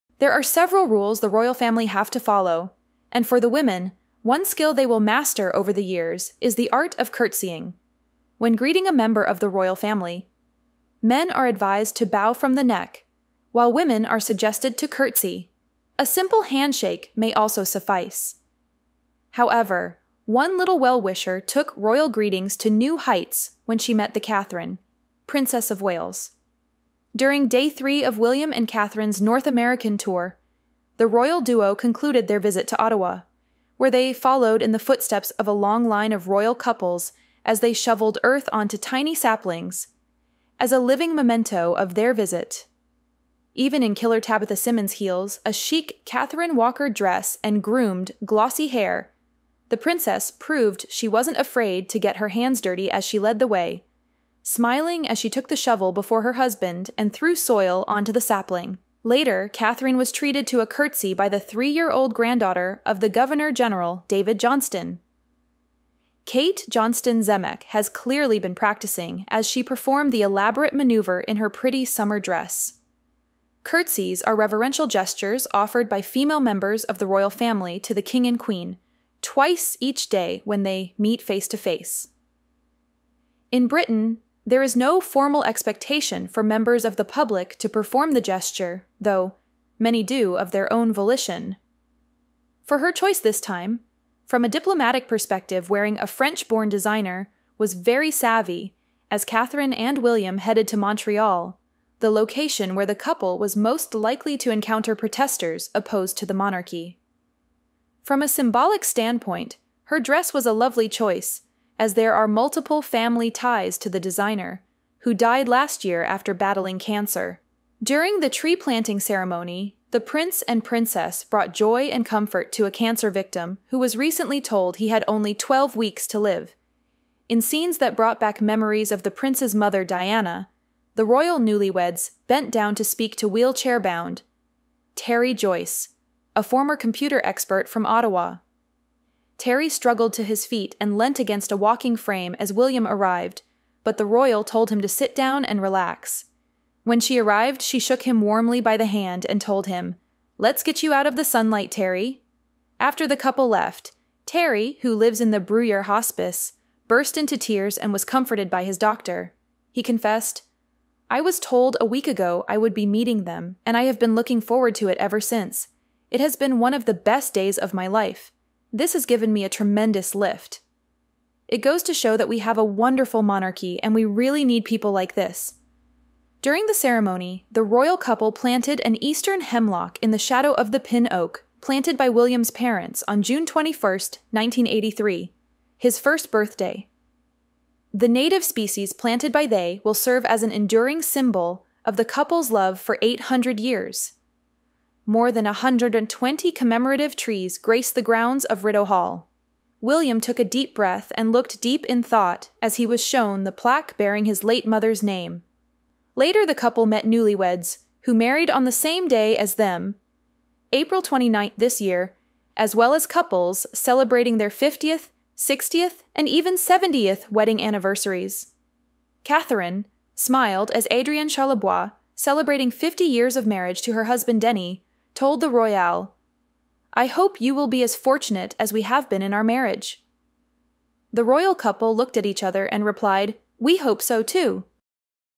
MELTING FANS! Princess Catherine Got VERY SWEET Curtsy From A Pretty Girl On Royal Tours
During the ceremony, the royal couple planted an Eastern Hemlock in the shadow of the Pin Oak planted by William's parents on June 21, 1983 - his first birthday. The native species planted by they will serve as an enduring symbol of the couple's love for 800 years. More than 120 commemorative trees grace the grounds of Rideau Hall.